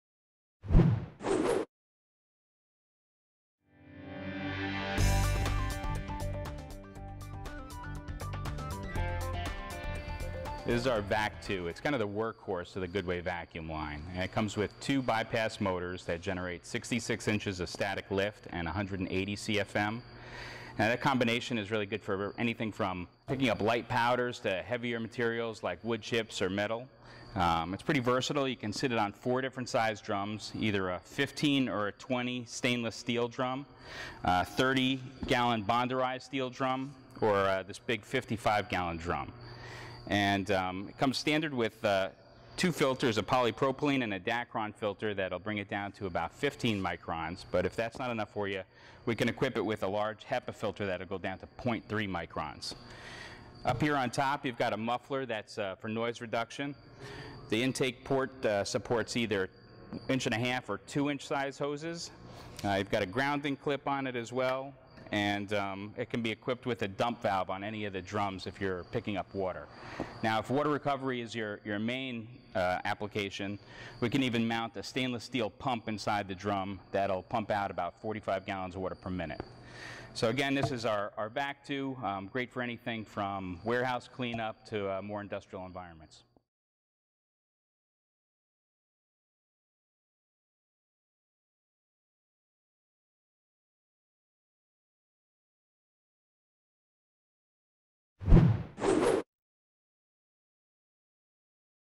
Goodway VAC 2 Wet Dry Industrial Vacuum
Enjoy the versatility of wet-dry performance. The VAC-2-HEPA Wet-Dry Industrial Vacuum is the safe solution for fine particle recovery applications. Equipped with twin-motor power, the VAC-2-HEPA produces a static water lift of 83" and an air displacement of 150 CFM to tackle even the toughest of tasks. How does it work? When recovering dry debris, the lower intake is used and a disposable paper filter bag traps the majority of material - capacity 5 gallons. When recovering liquids or bulk materials, the upper intake is used - capacity 12 gallons. In all cases, fine particles are filtered to 0.3 microns by a cloth filter, a Dacron filter, a polypropylene filter and a HEPA filter. Only "clean", safe air passes through the vacuum exhaust. Version available with vacuum head and drum ring to attach directly to 55 gallon drum.